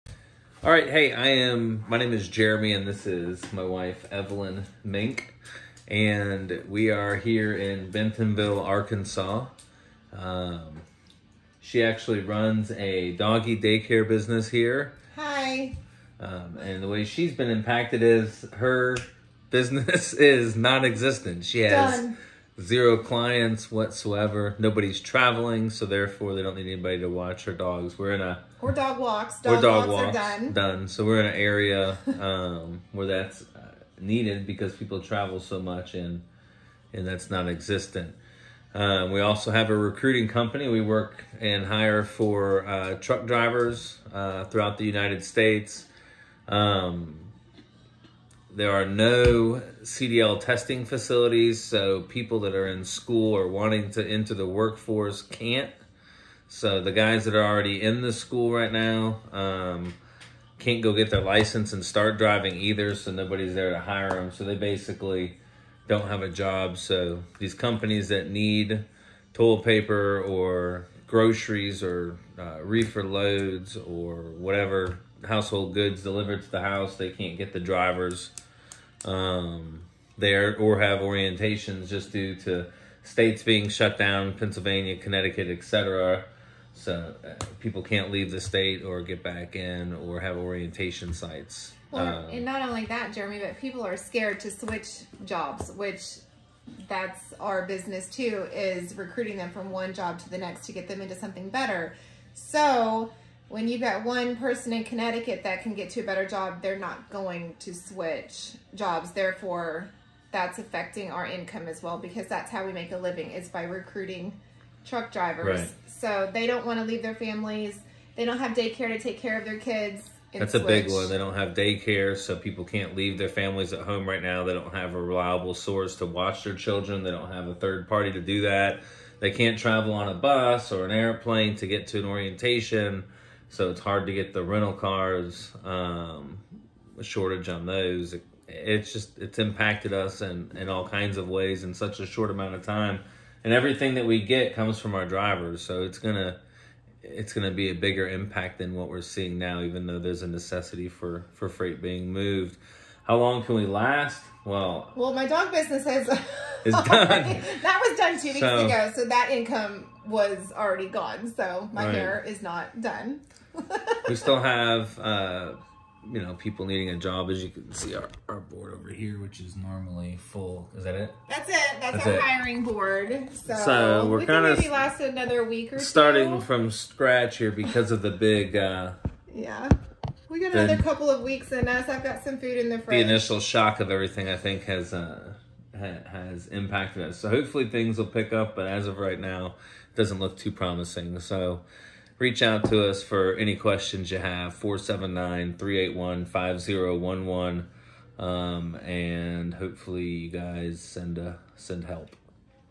WHY AMERICA NEEDS TO GET BACK TO WORK | Stories from ACTUAL BUSINESS OWNERS | Recruiter
See what an ACTUAL small business owner has to say about why America needs to get back to work!